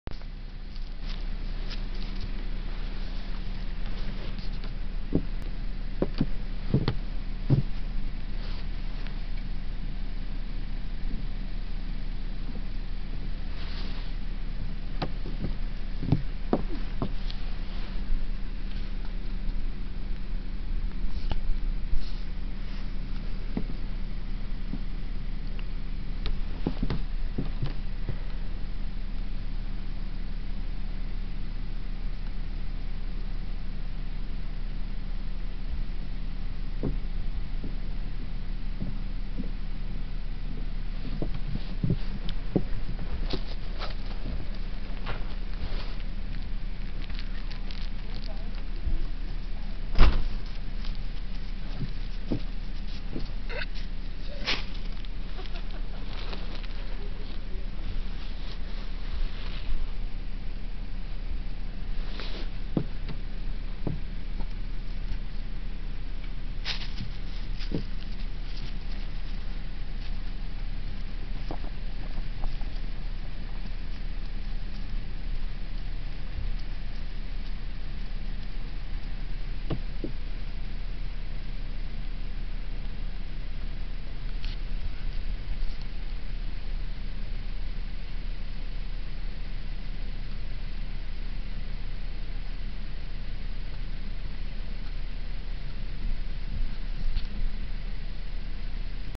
Christmas Donkey
Jessy got loose and we dressed him up for the holiday